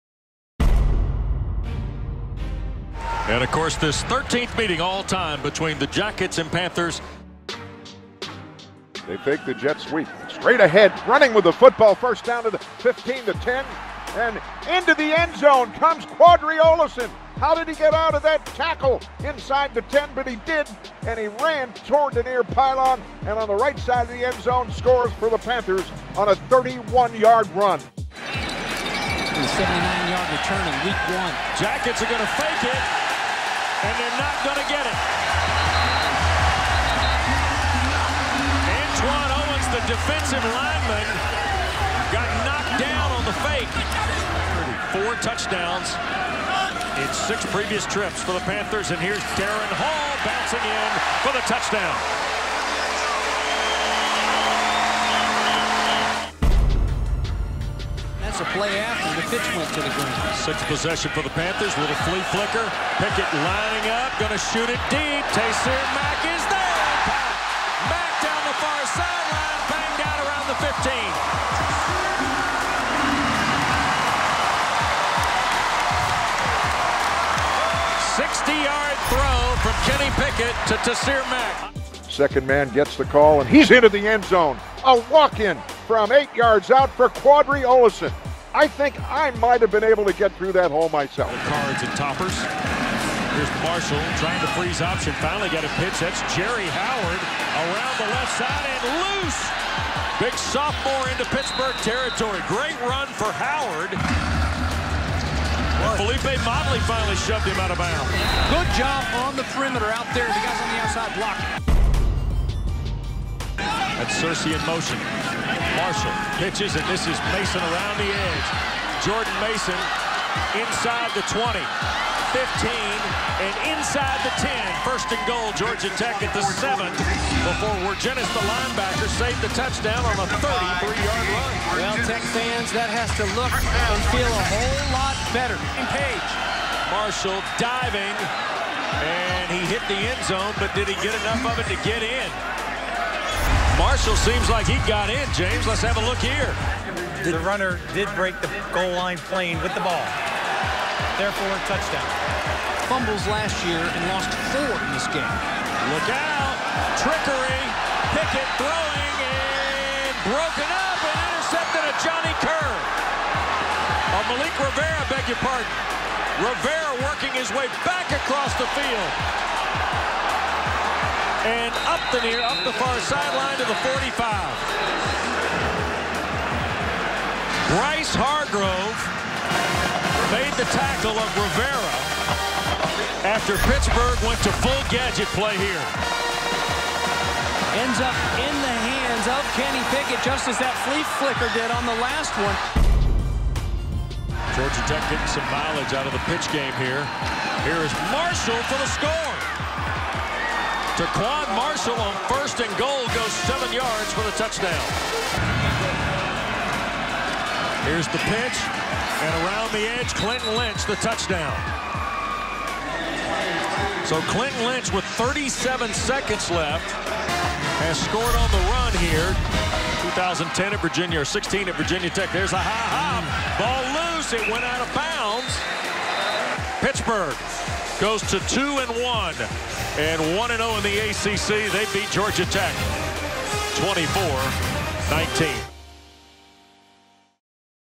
Georgia Tech vs. Pittsburgh Football Highlights (2018)
Georgia Tech vs. Pittsburgh: Panthers running back Qadree Ollison rushed for 91 yards and 2 touchdowns, while the Pitt defense forced two Yellow Jackets turnovers in a 24-19 win Saturday afternoon at Heinz Field. Quarterback Kenny Pickett threw for 197 yards in the win. Georgia Tech's Taquon Marshall rushed for 103 yards and 2 scores.